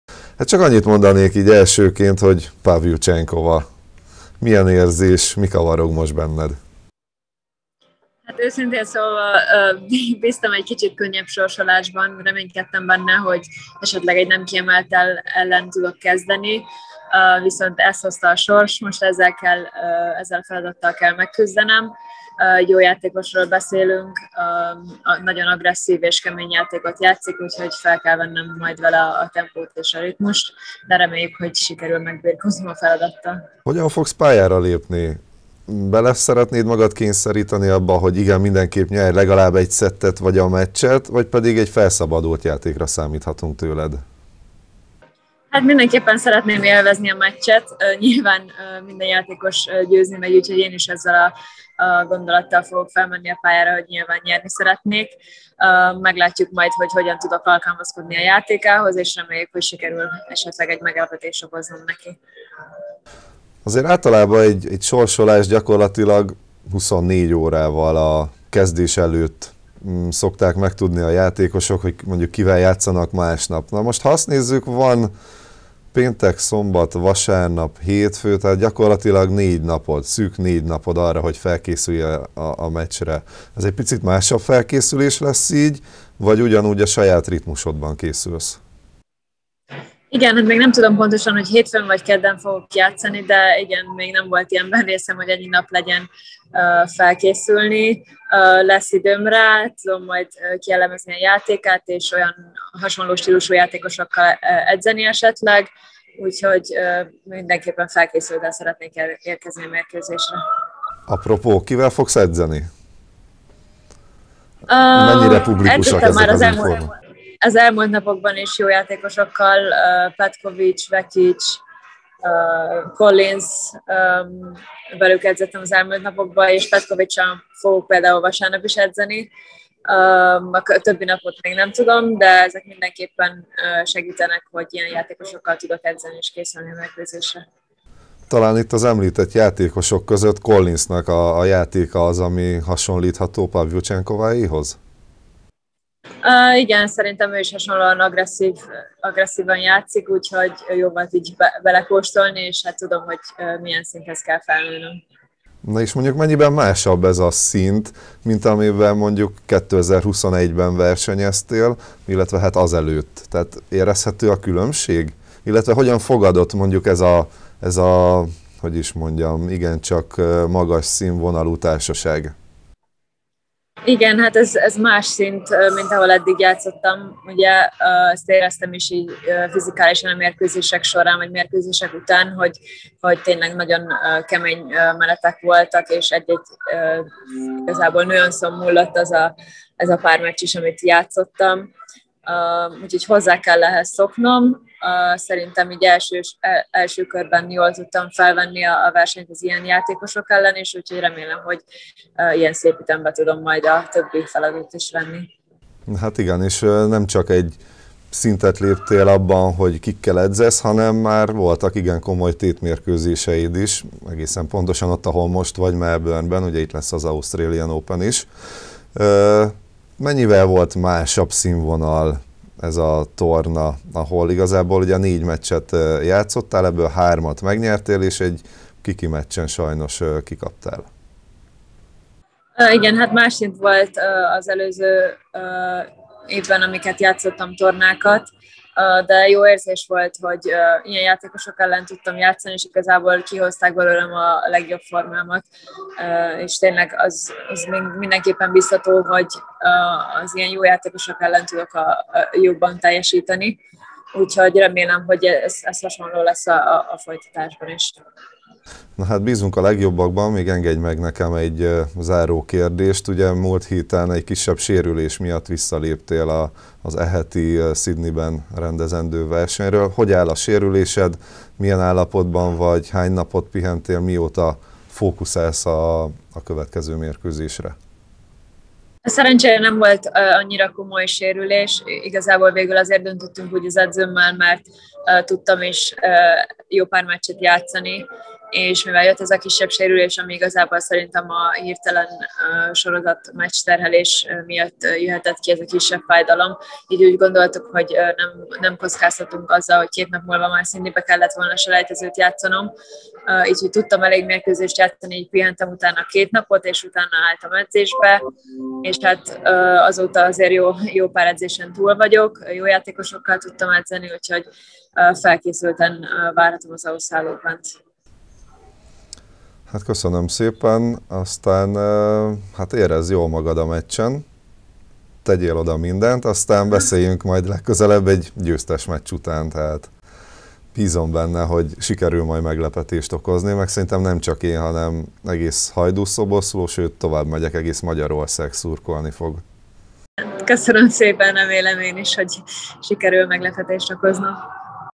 Bondár Anna: Szeretnék meglepetést okozni
A 10. helyen kiemelt Anastasia Pavlyuchenkova ellen játszik a HSE teniszezője az Australian Open első fordulójában. A HVTV-nek azt mondta, hogy egy agresszív és kemény játékossal játszik. Élvezni szeretné a meccset és meglepetést okozna a nyitófordulóban. Hogy kikkel készül Bondár Anna, nézze meg a HVTV-nek adott interjúját!